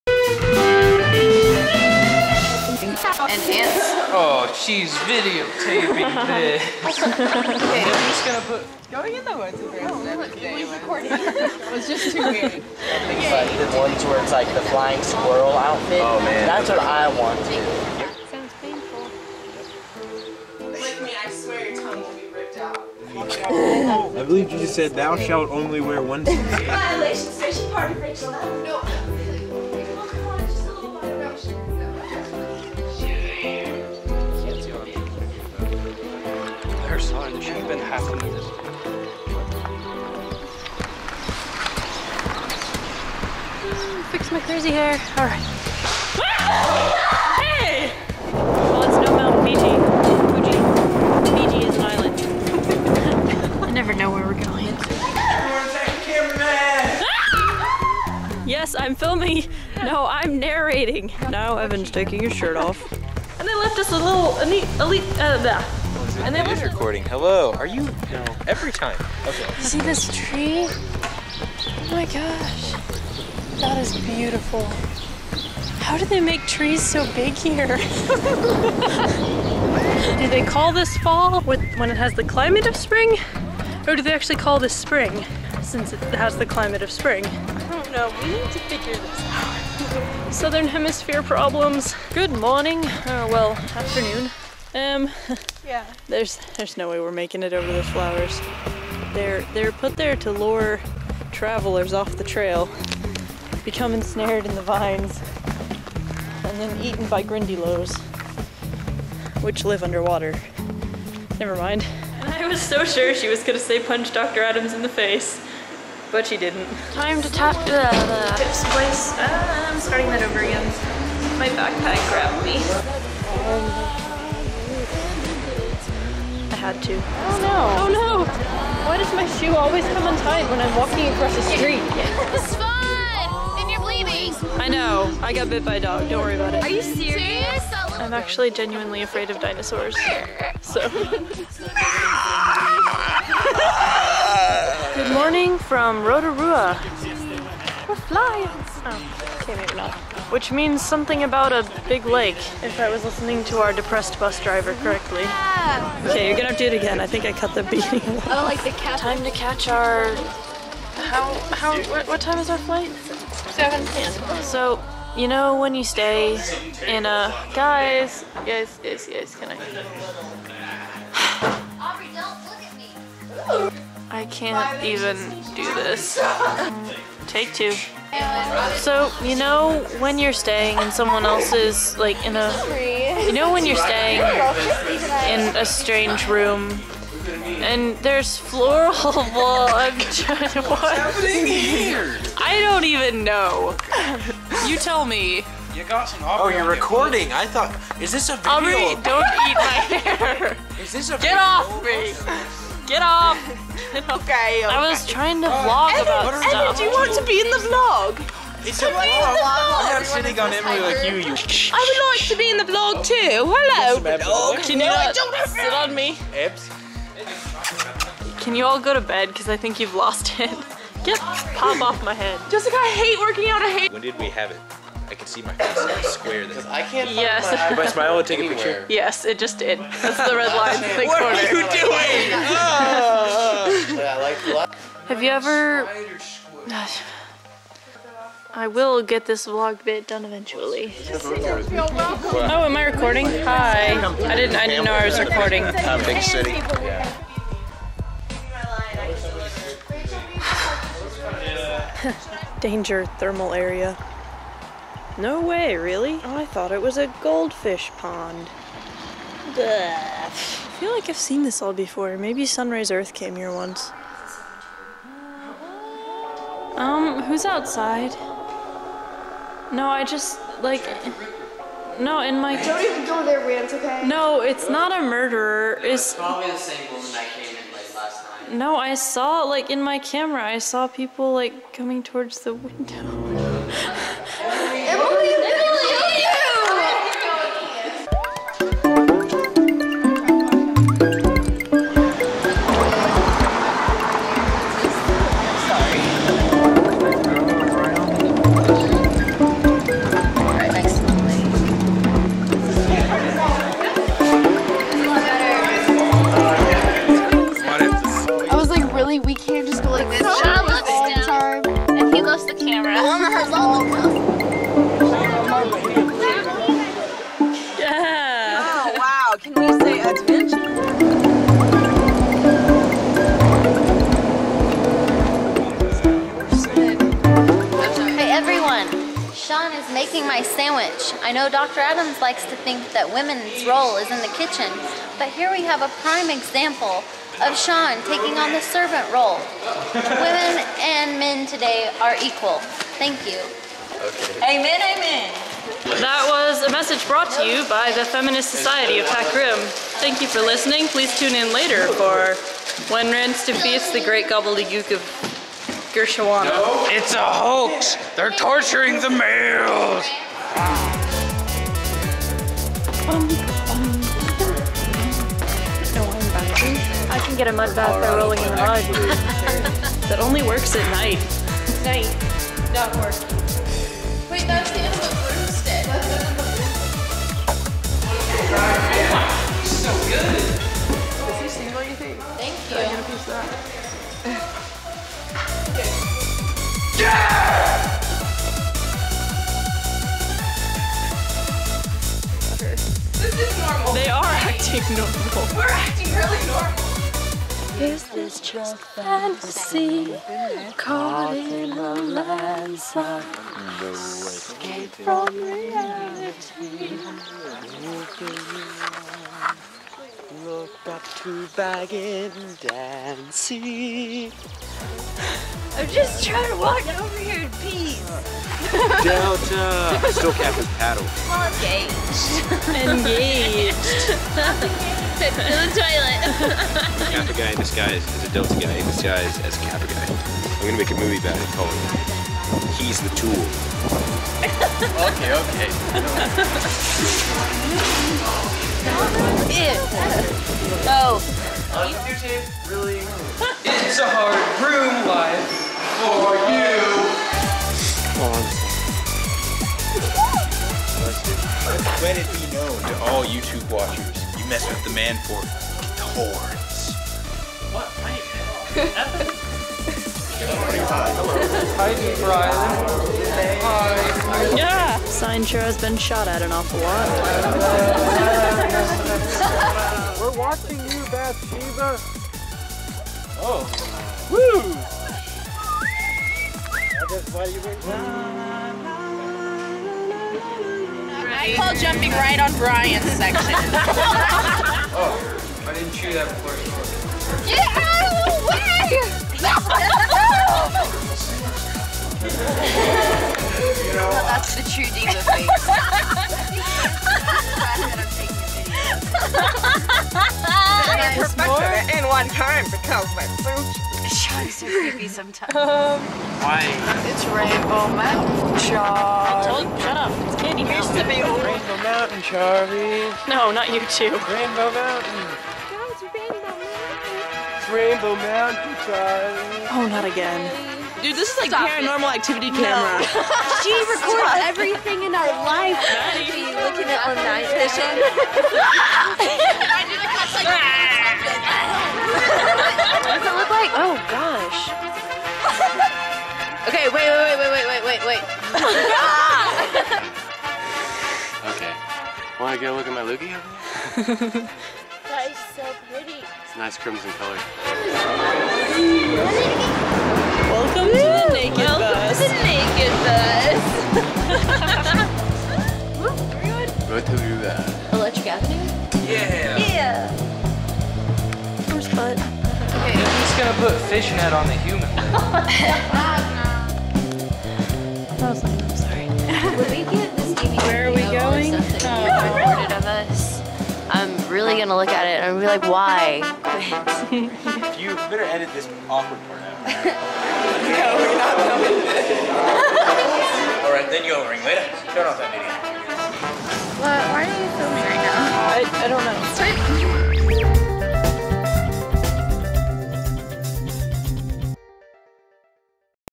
PAC RIM: Bloopers [100% New Zealand Made]
The bloopers and extra scenes from two weeks in New Zealand on PAC RIM. From Auckland to the Maori village, there were so many funny moments I had to make a whole extra video.